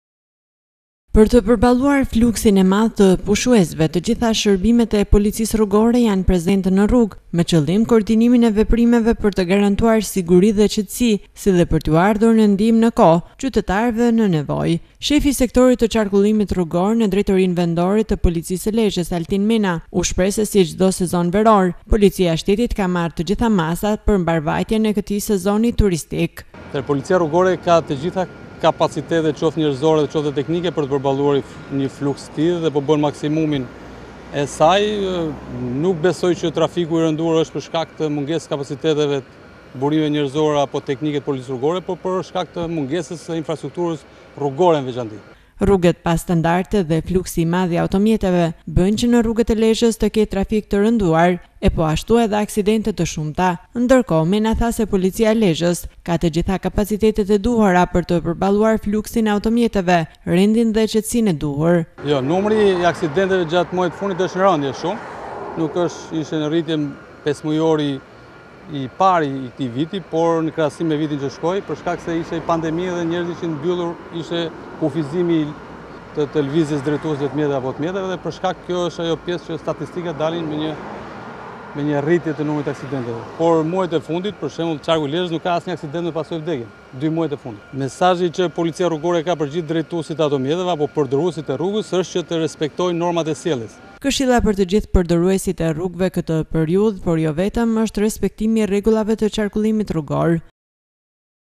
POLICIA RRUGORE DHE PLANI I MASAVE PER PERBALLIMIN E SEZONIT TURISTIK
Njesia e qarkullimit rrugor ne drejtorin e policise se qarkut te Lezhes ka marre te gjitha masat e nevojshme per mbarevjatjen e sezonit turistik. Shefi i kesaj njesie ne Lezhe Altin Mena, shprehet se rruget pa standarte dhe fluksi i larte i automjeteve krijojne veshtiresi te shumta, duke cilesuar faktin se policia e lezhes ka te gjitha kapacitetet e nevojshme per te perballuar trafikun, rendin dhe qetesine publike.